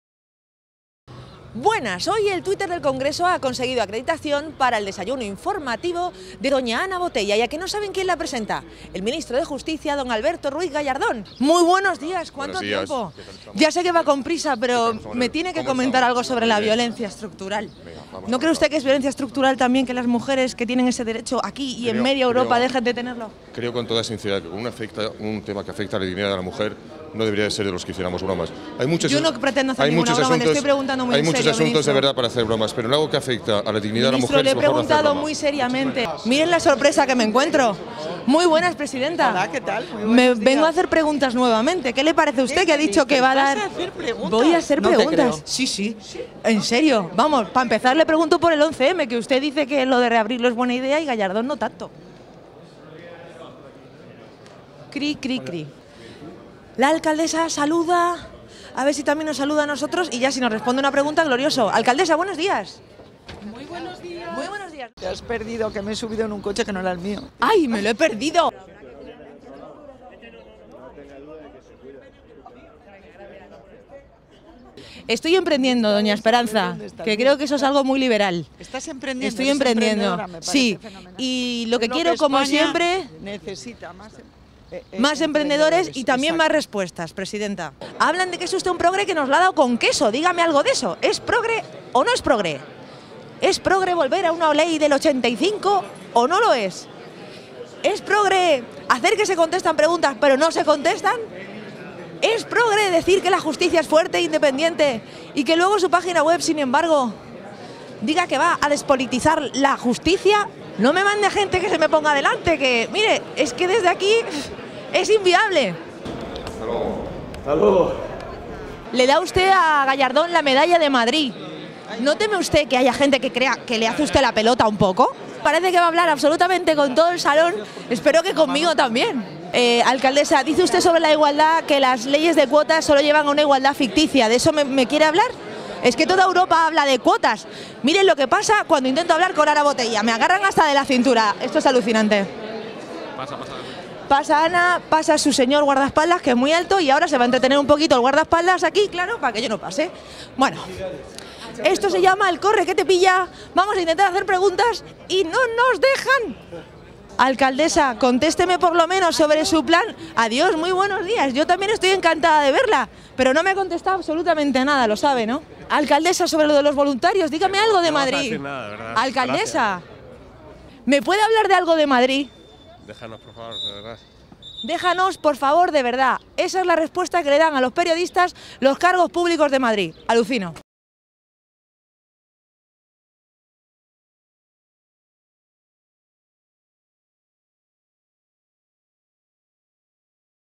DESAYUNO ¿INFORMATIVO? DE ANA BOTELLA
Un reportaje surtidito que contiene: "El Show de Ana", "Momentos Espe" y "En Busca del Progre Perdido"... Todo un espectáculo desinformativo.